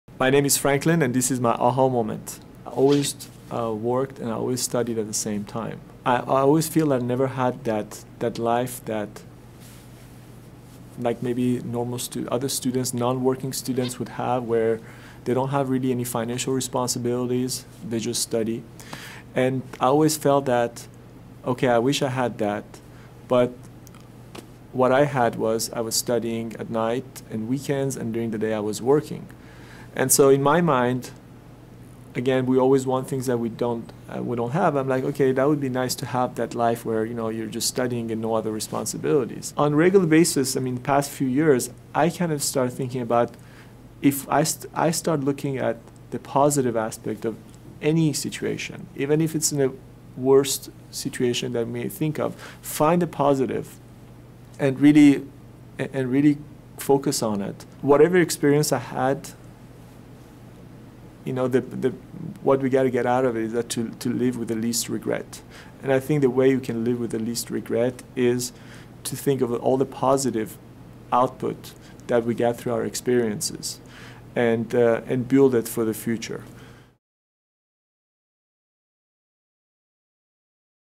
Franklin Mohri's personal aha moment
The only way to succeed in this business is to genuinely care about the people you work with, and do your best to try to help them every single day. I'm fortunate that my clients have put their trust in me to protect their families, and it's something that I take very seriously.